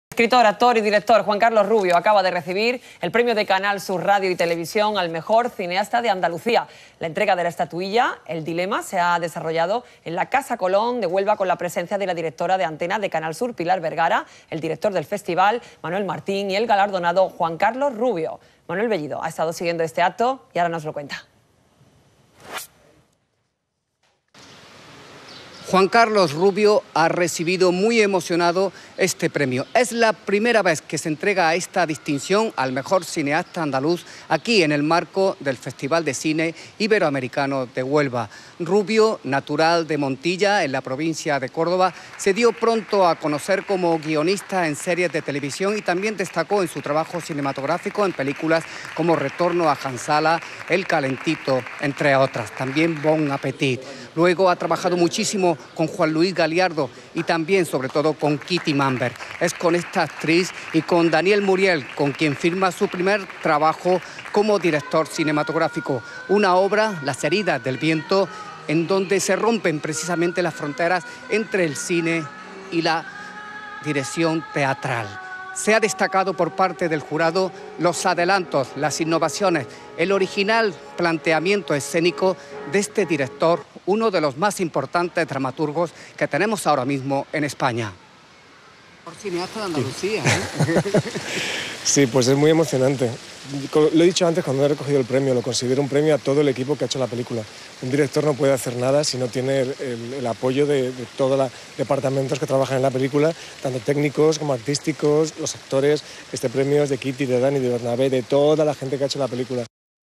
Canal Sur entrega al montillano Juan Carlos Rubio el premio al Mejor Cineasta de Andalucía
El periodista, también montillano, Manuel Bellido, que cubrió la noticia para el informativo de la cadena autonómica, le calificó como "uno de los más grandes dramaturgos que tenemos ahora mismo en España".